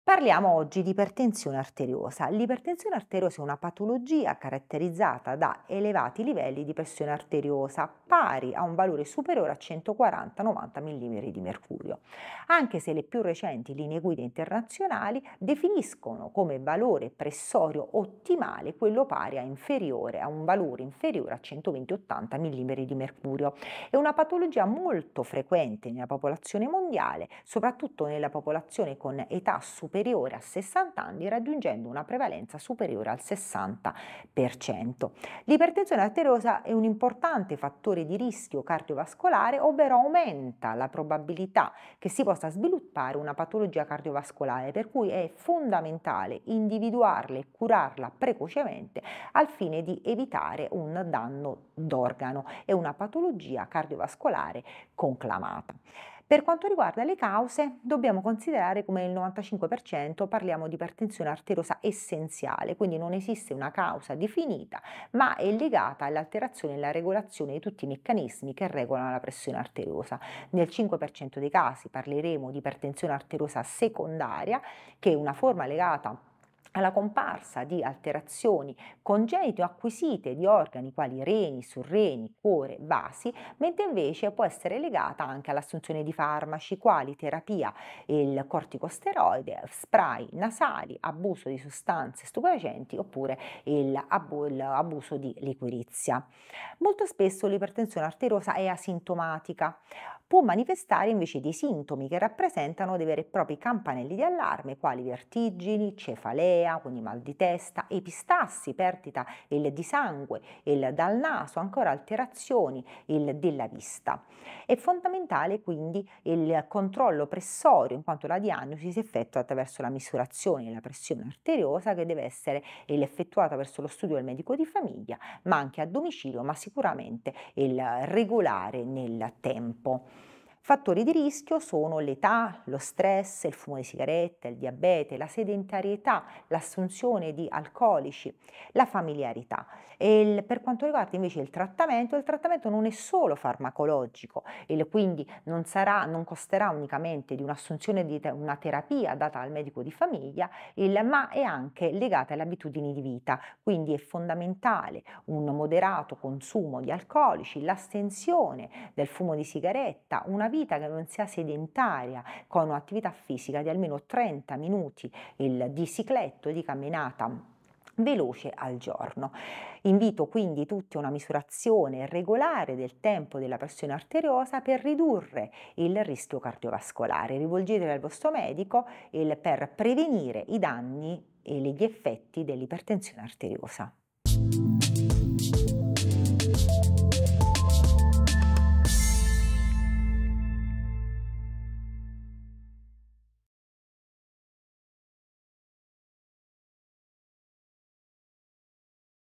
FADOI per la Prevenzione - Ipertensione Arteriosa
La Prevenzione dell'Ipertensione Arteriosa: Prenditi Cura del Tuo Cuore

L'ipertensione arteriosa, comunemente conosciuta come pressione alta, è una condizione che colpisce milioni di persone in tutto il mondo. È un fattore di rischio principale per malattie cardiovascolari, ictus e insufficienza renale. Fortunatamente, ci sono molte strategie che possiamo adottare per prevenire e gestire l'ipertensione.

Ecco alcuni consigli pratici per mantenere il nostro cuore in salute: a parlarcene è la Dott.ssa Maria Gabriella Coppola - Direttore UOC Medicina d’Urgenza e Pronto Soccorso AORN “San Pio” di Benevento.

Prevenire l'ipertensione arteriosa richiede un impegno continuo, ma i benefici per la tua salute e il tuo benessere generale ne valgono sicuramente la pena.

❤️ Prenditi cura del tuo cuore oggi per un futuro più sano!

FADOI - Società Scientifica di Medicina Interna